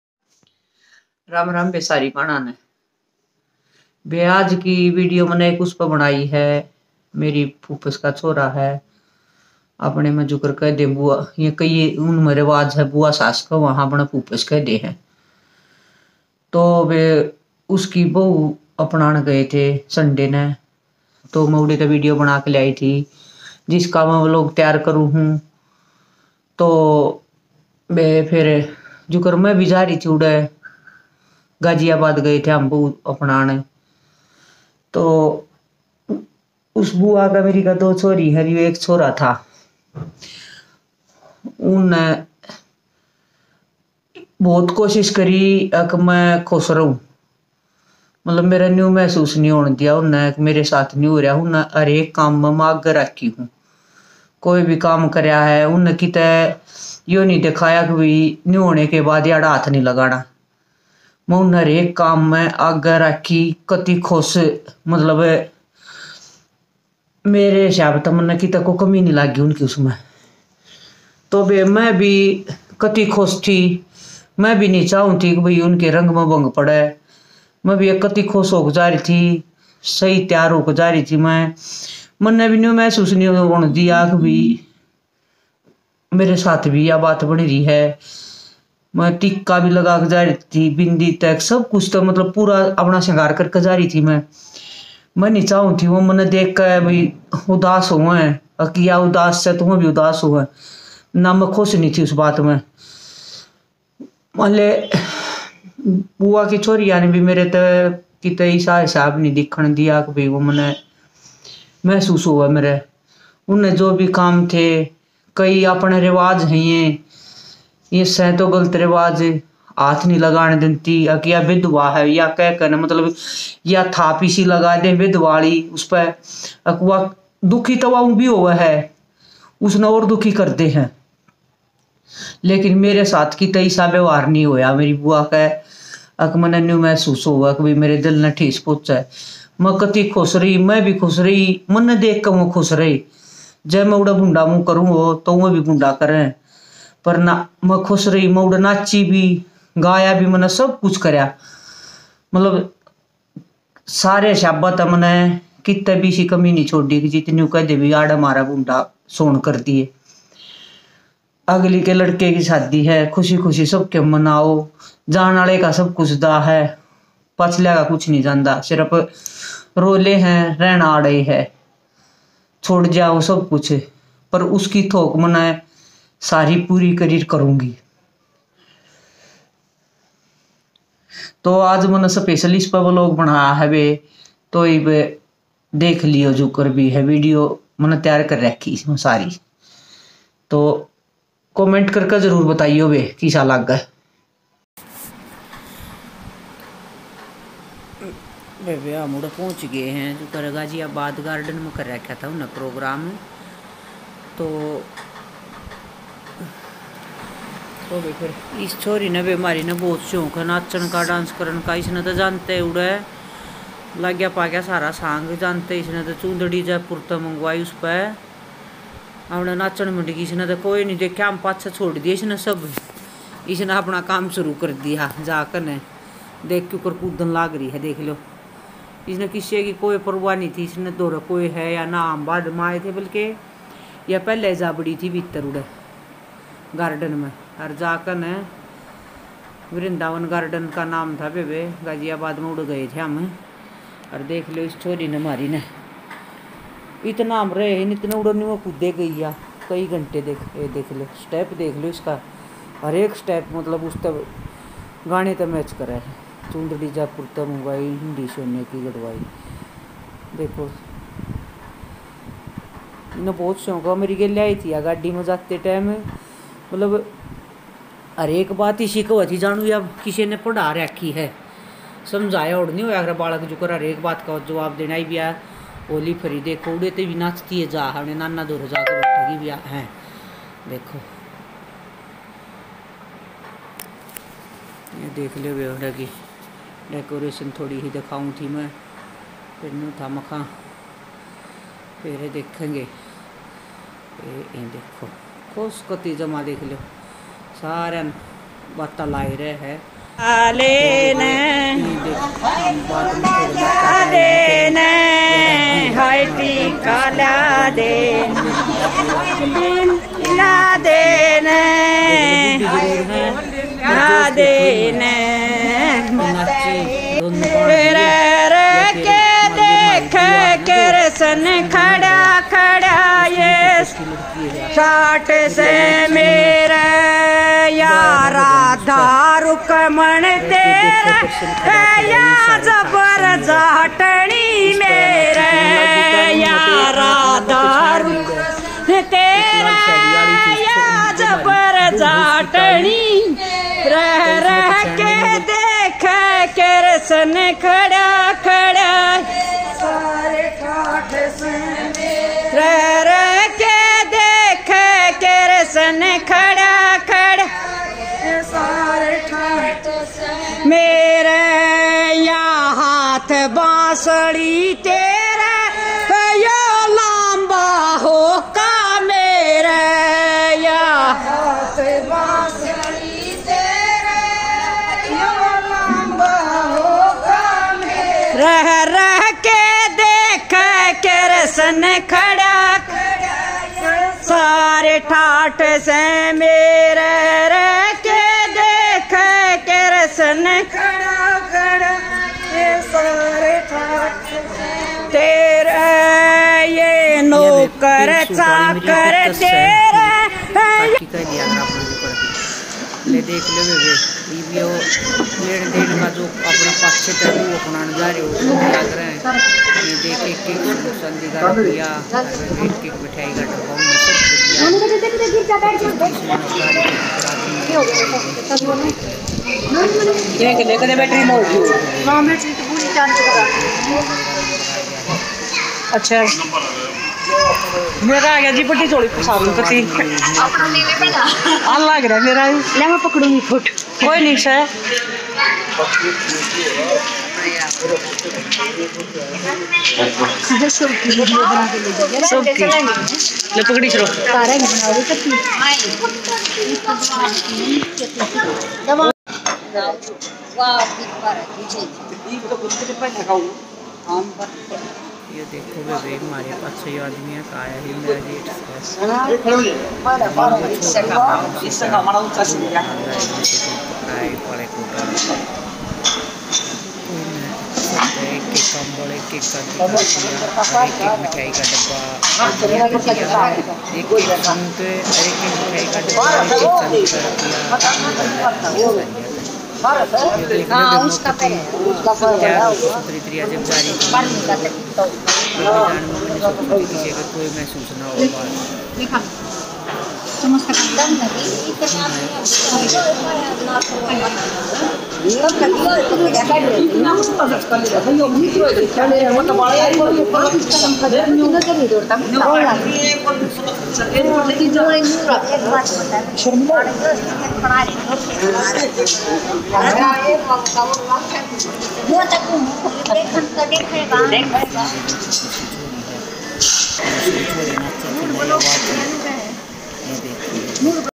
जब हम देवर की बहु अपनाने गए तो 🌹🌹मेरे साथ कैसा व्यवहार हुआ 🌹🌹(5)
पपल रापरिया सांगवान चैनल की तरफ से सभी को राम राम जी 🌹🌹